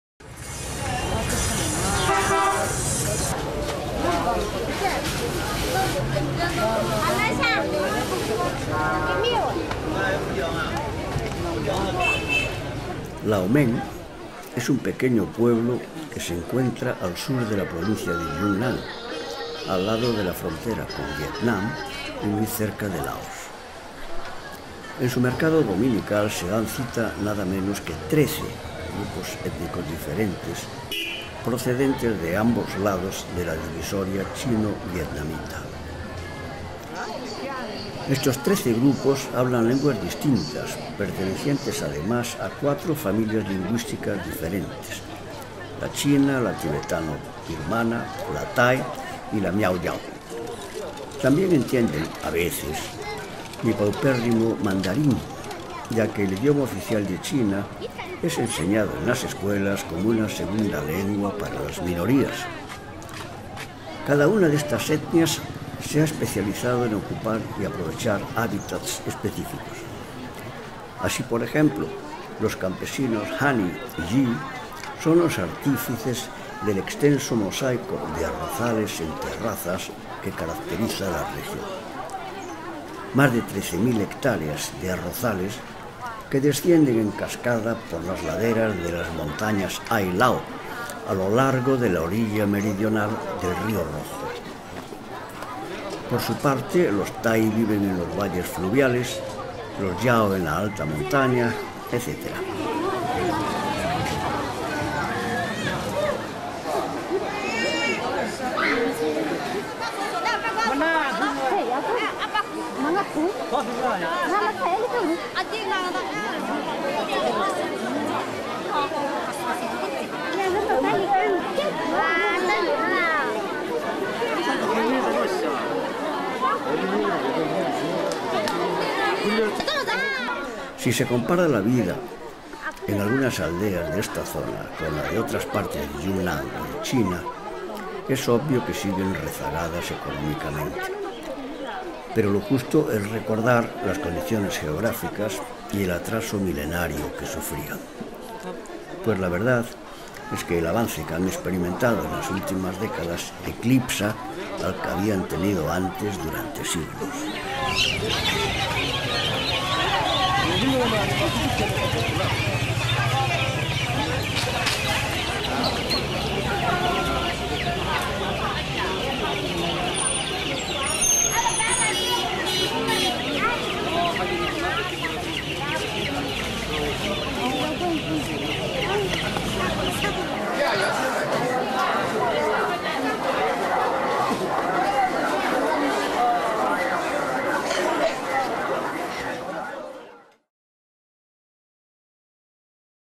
Laomeng 老孟 (R.P. China): el mercado con 13 etnias - the market with 13 ethnic groups
Laomeng 老孟  -  Yunnan 云南 -  China 中华人民共和国

Grabado y editado en 2009.
Recorded and edited in 2009.